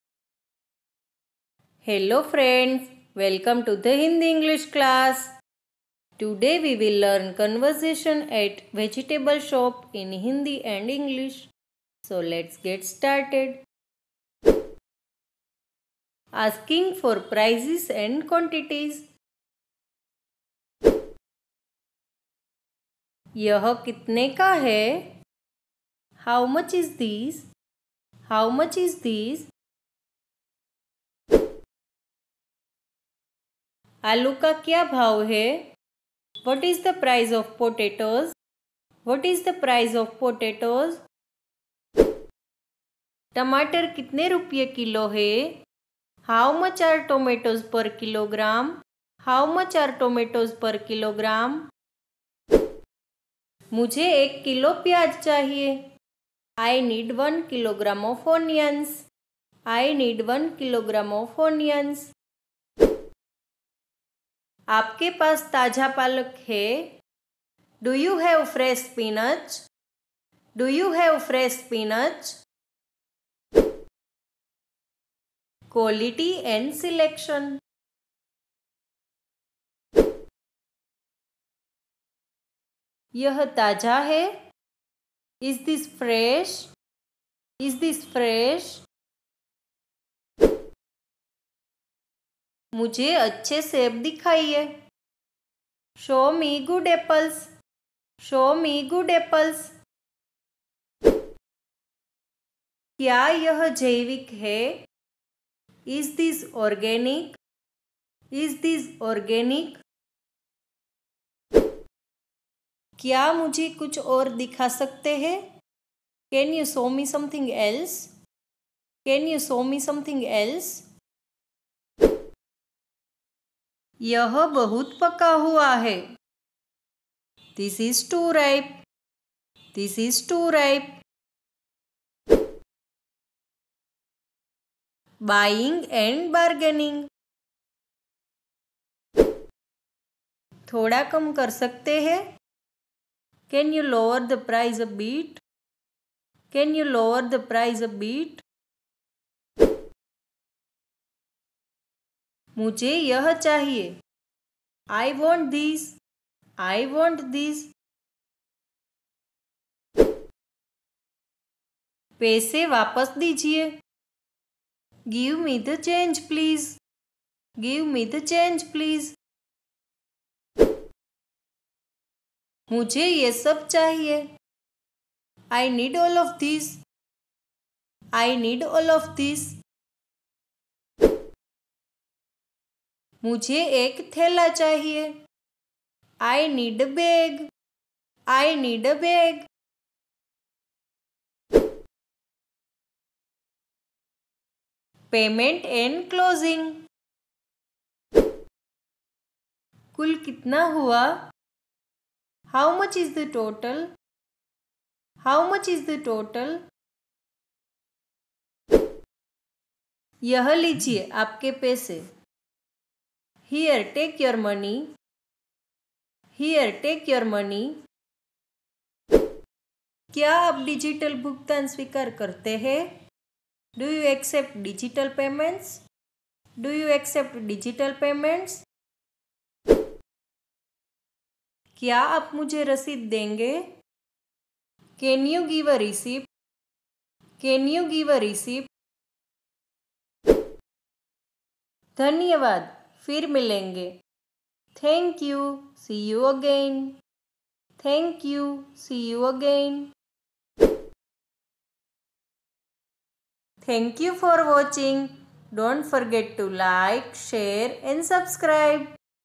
English Conversation at Vegetable Market | Shopping at the Grocery Store - English Conversation
Learn important words and phrases that you can use at the grocery store in this video.